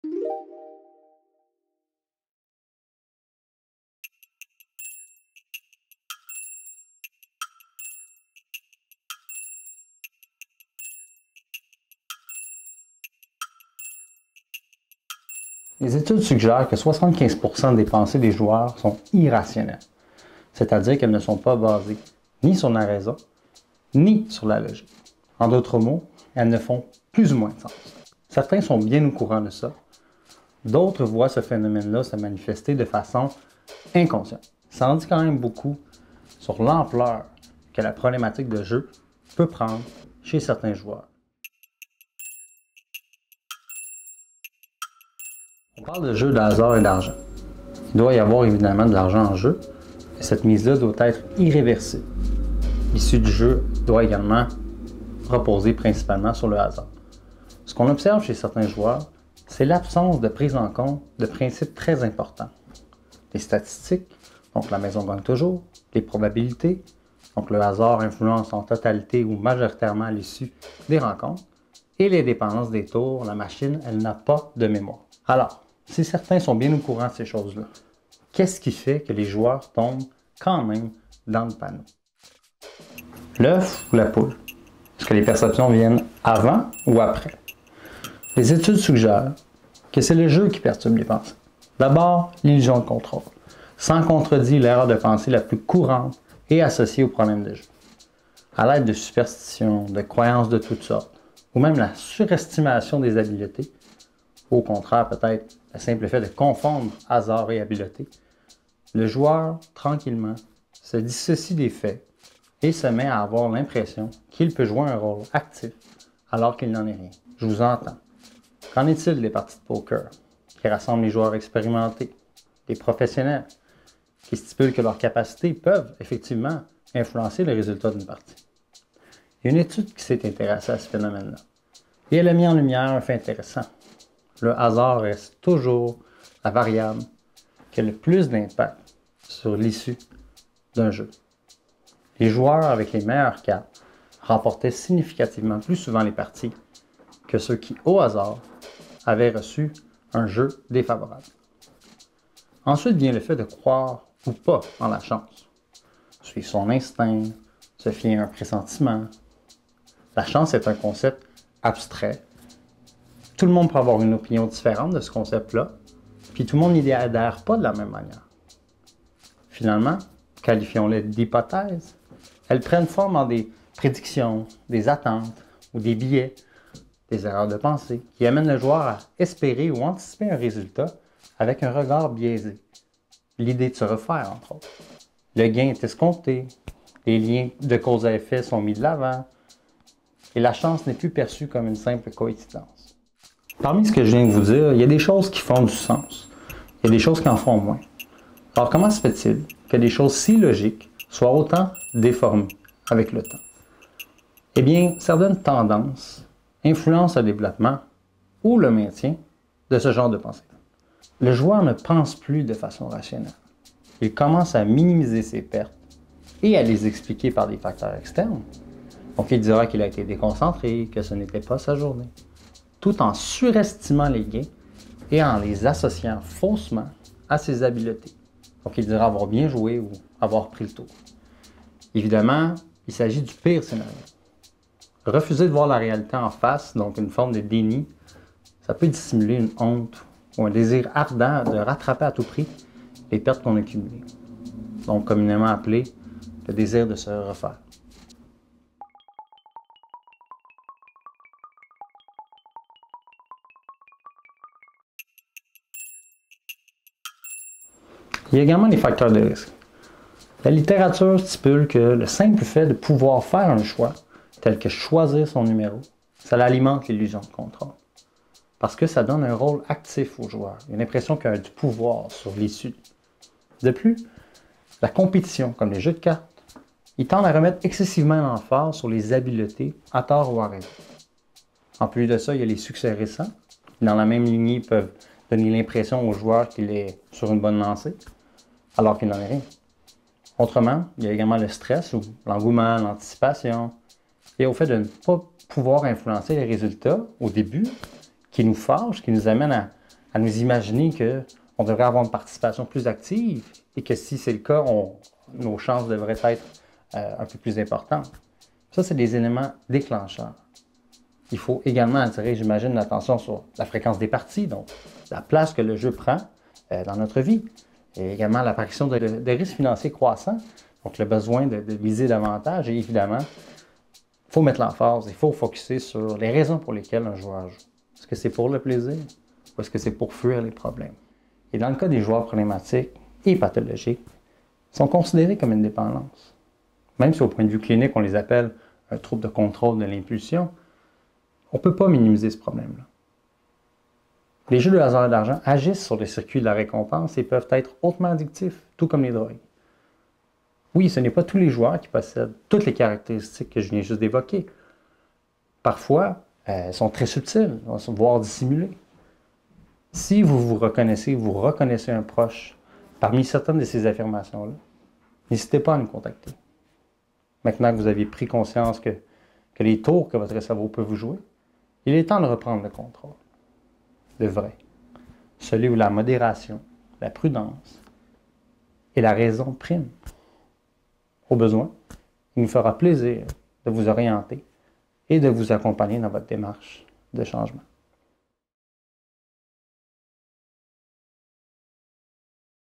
Les jeux de hasard et d'argent jouent-ils plutôt avec vos pensées?
Quelles images vous viennent en tête lorsqu'on vous parle de jeux de hasard et d'argent? Casino, loterie ou alors serait-ce plutôt le simple fait d'être chanceux? Quelles que soient vos croyances, votre relation avec le jeu peut rapidement se dégrader si l'excès et les superstitions prennent le contrôle sur vous.

Dans cette capsule, vous apprendrez les mécanismes à l'origine du joueur problématique ou pathologique. Si la plupart sautent aux yeux, certains sont beaucoup plus malicieux..

Vous vous reconnaissez ou reconnaissez un proche? N'hésitez pas à nous contacter.

- Sébastien Latendresse,
Coordonnateur et Intervenant